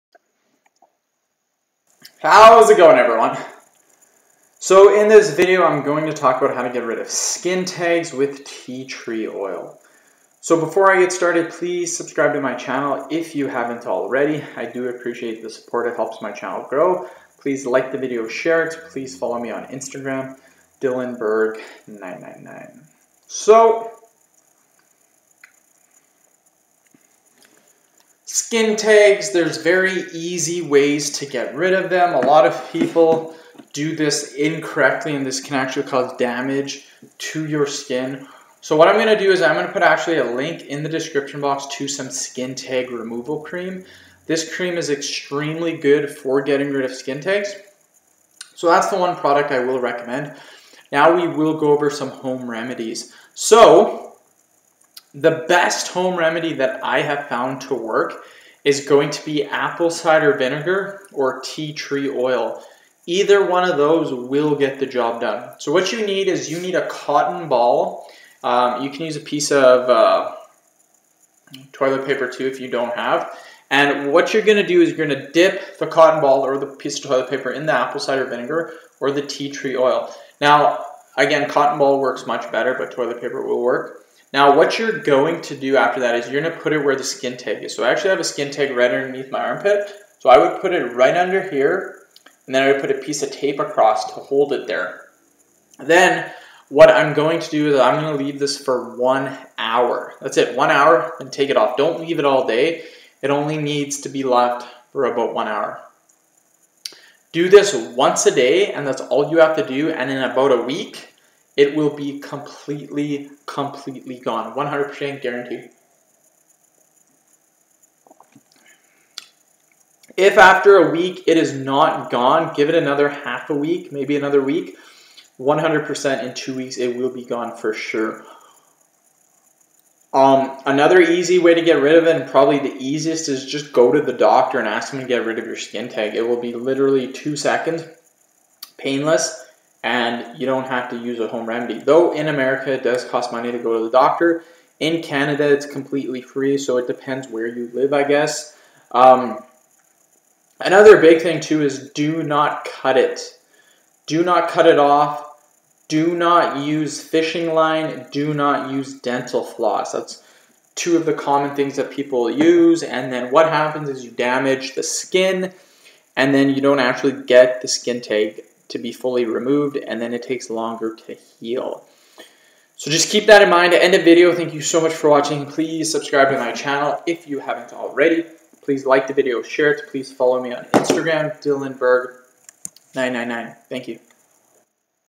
how to get rid of skin tags with tea tree oil
how to get rid of skin tags with tea tree oil. Here in this video i talk about how to get rid of skin tags with tea tree oil.